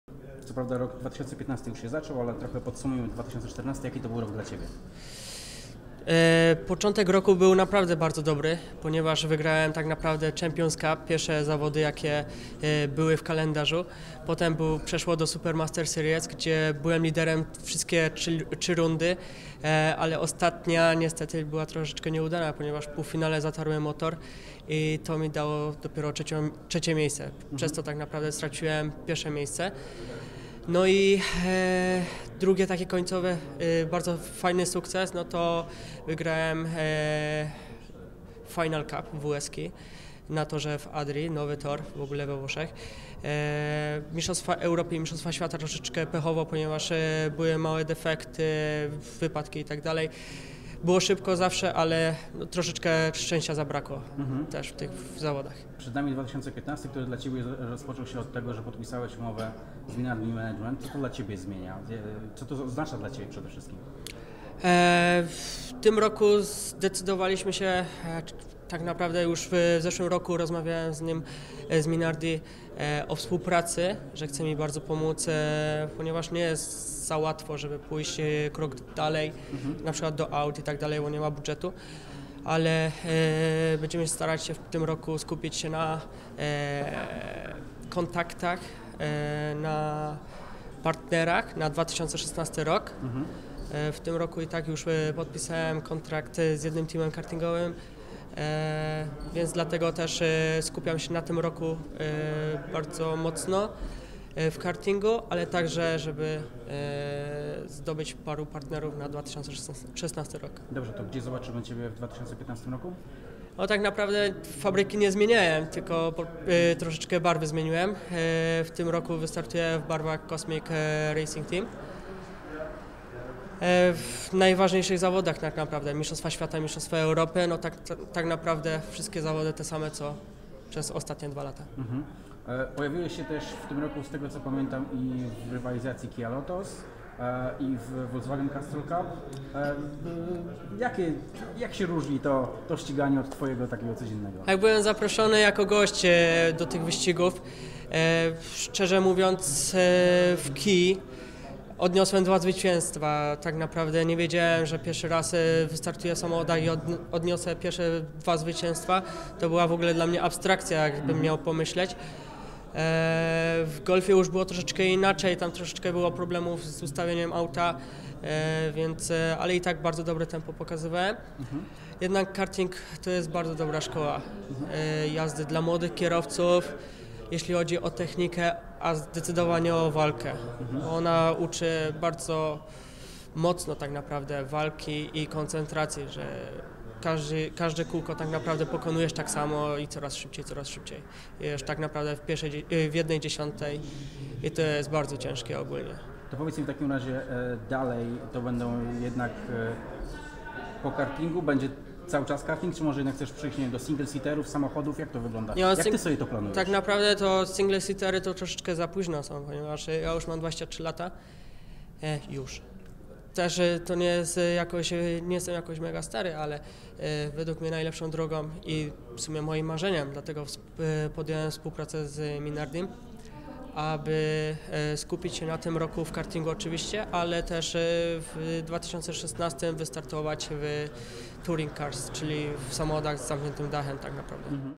WrOOOOm Tv - Rozmowa z Karolem Baszem
Zmiana barw, nowa firma managerska, wielkie plany i lekki niedosyt po sezonie 2014. To w skrócie ostatnie dni Karola Basza. O planach opowiedział nam sam zawodnik podczas Gali Sportu Kartingowego.